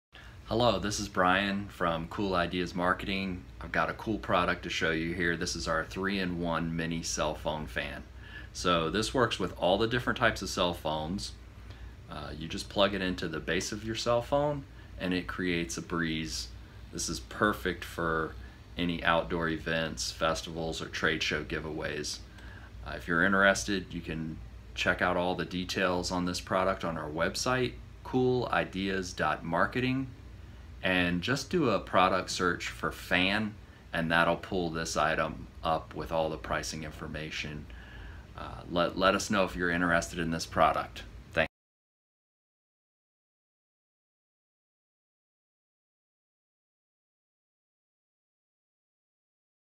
3-in-1 Mini Phone Fan
Grow your business with this cool promo product! 3-in-1 Mini Phone Fan 3 adaptors to fit your Android, Apple or Type-C devices Rubber blades, Soft-touch casing. Complies with Prop 65. Price and details at coolideas.marketing (no .com).